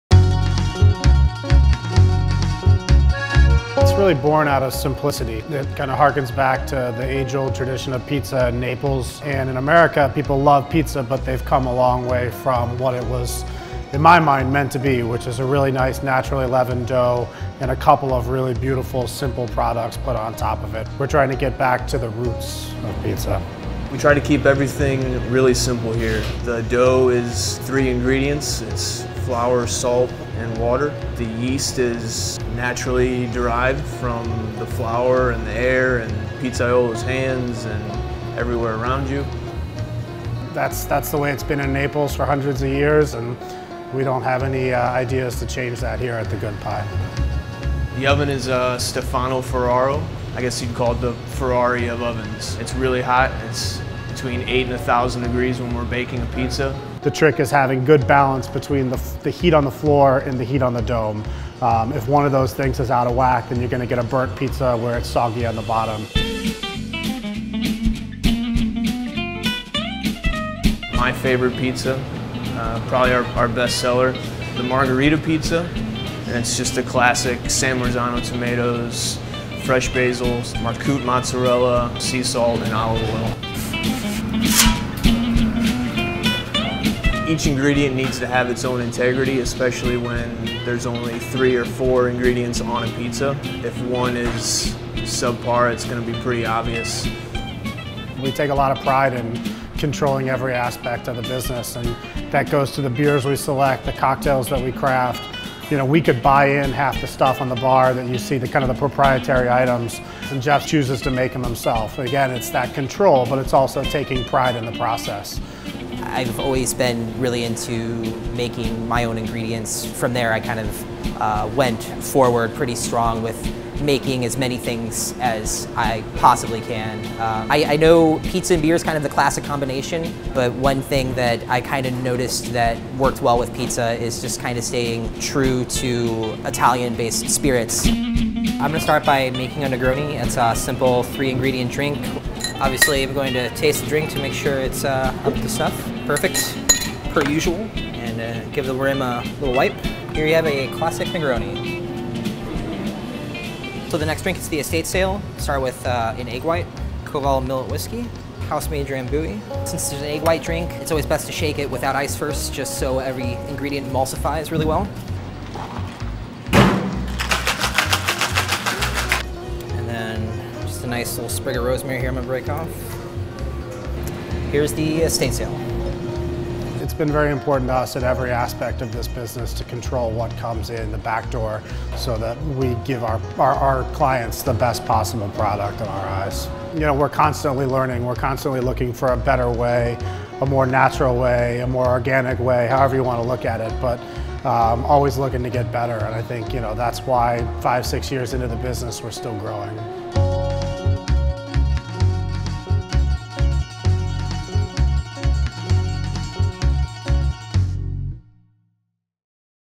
Behind the Scenes: The Good Pie
Mike Randolph, chef/owner at The Good Pie, says the pizza here is born out of simplicity, drawing back to the age-old tradition of pizza-making in Naples. We take a look at The Good Pie's pizza-making process, from the three ingredients that make up the dough to the fresh toppings to the traditional Neapolitan brick oven.